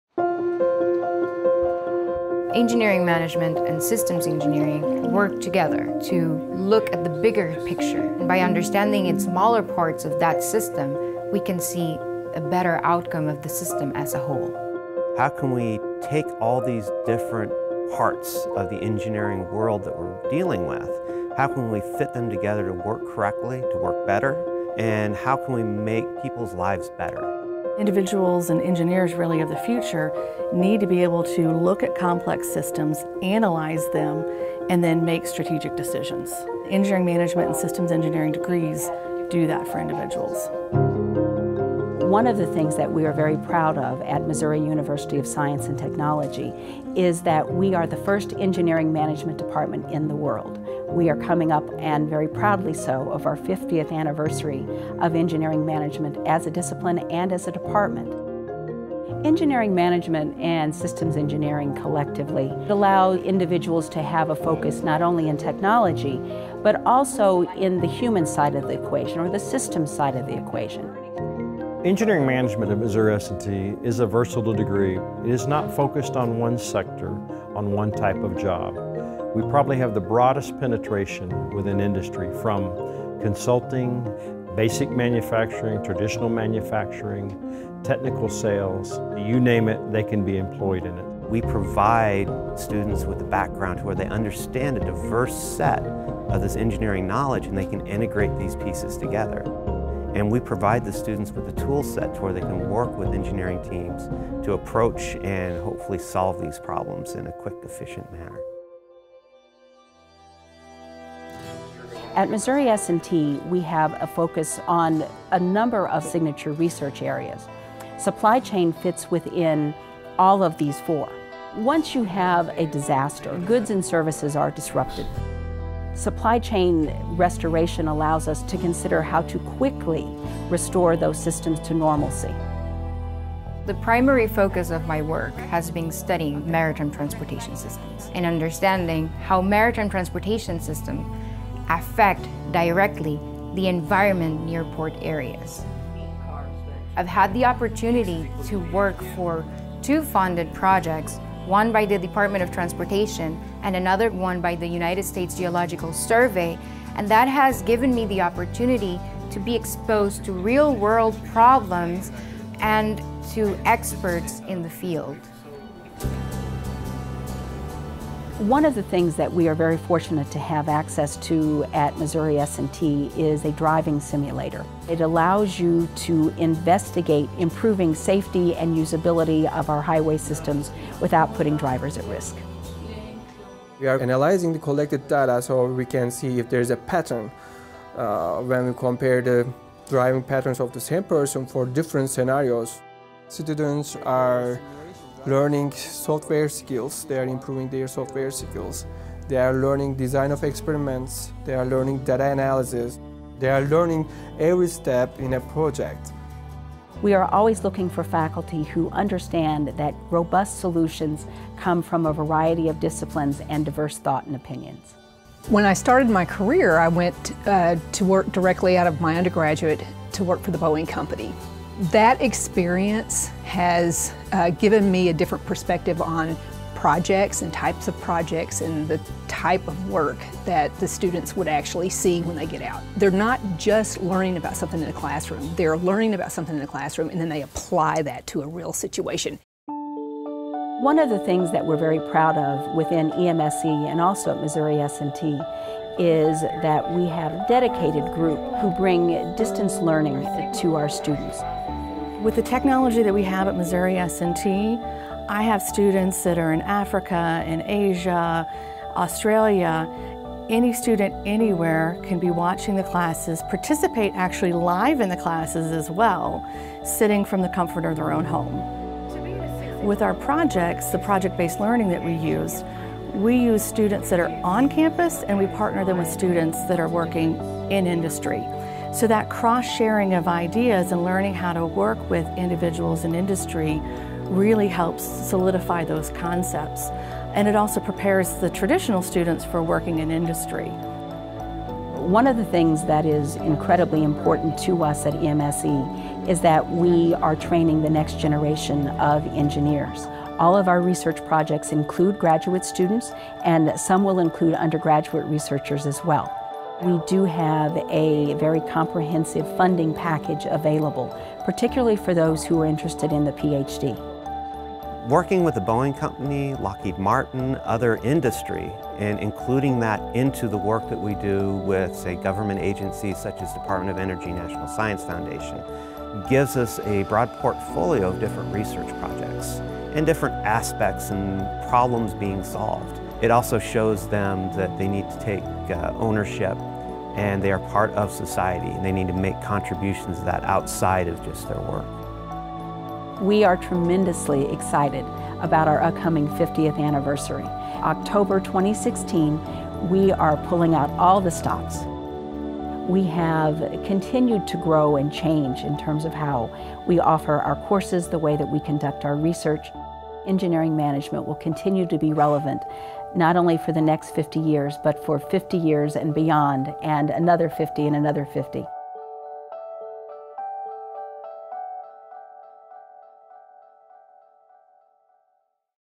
Missouri S&T: Department of Engineering Management & Systems Engineering (EMSE)
Missouri University of Science and Technology is integral to the history of Engineering Management. It was the university’s own Professor Bernie Sarchet who invented the term 50 years ago and the department has grown in significance ever since. Offering a wide-ranging engineering management degree to undergraduates, as well as masters and PhD programs in both Systems Engineering and Engineering Management, EMSE is a leader in finding innovative cutting edge ways to train the next generation of  systems engineers.